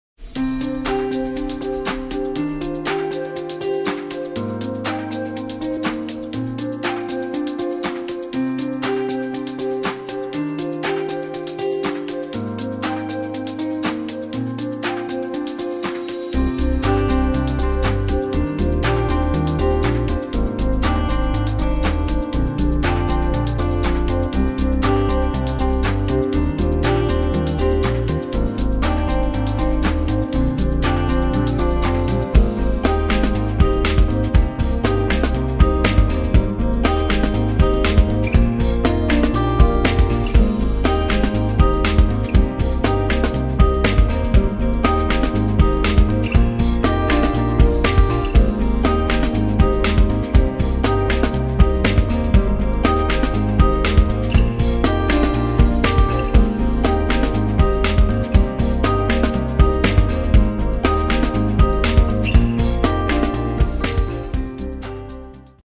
Thule SnowPack Extender síléctartó 7325, 62,5 cm
Aerodinamikus, elcsúsztatható síléctartó a sílécek könnyű fel- és levéleteléhez.
Rakodási szélessége 62,5 cm, 5 pár síléc szállítására alkalmas. Stílusos kialakítása minden típusú síléchez, snowboardhoz remekül illeszkedik. Ezüst színben.